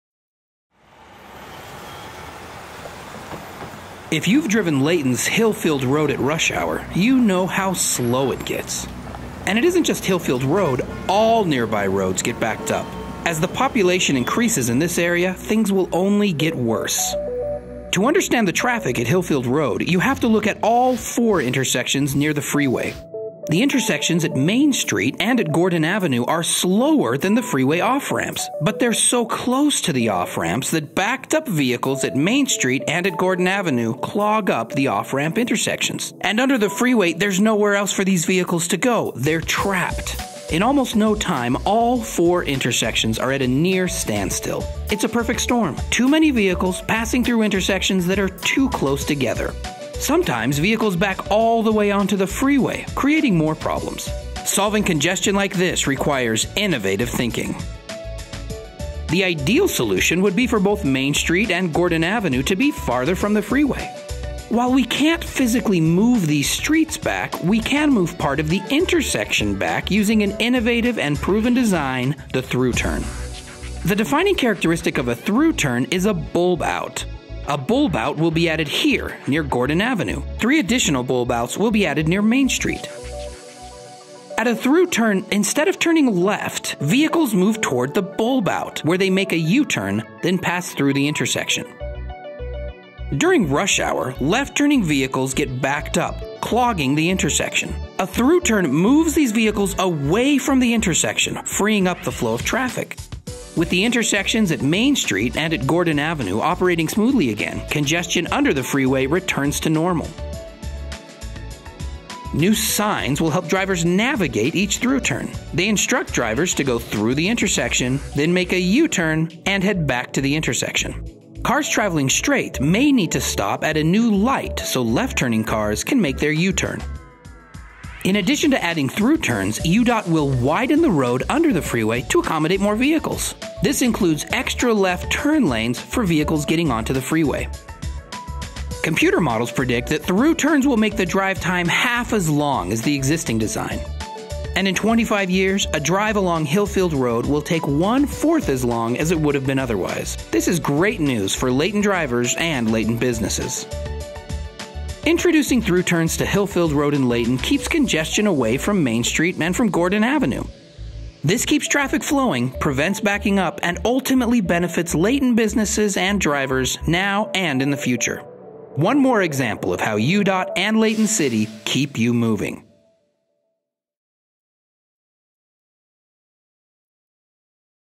ThrU Turn Overview, Layton, Utah
2015
Client: Utah Department of Transportation
Purpose: Describe ThrU Turns, and how they would be used on Hill Field Road in Layton, Utah.
This video was accompanied by tutorials for specific regions: Gordon Avenue and Main Street as well as a POV comparison of the non-ThrU-Turn route with the new ThrU-Turn route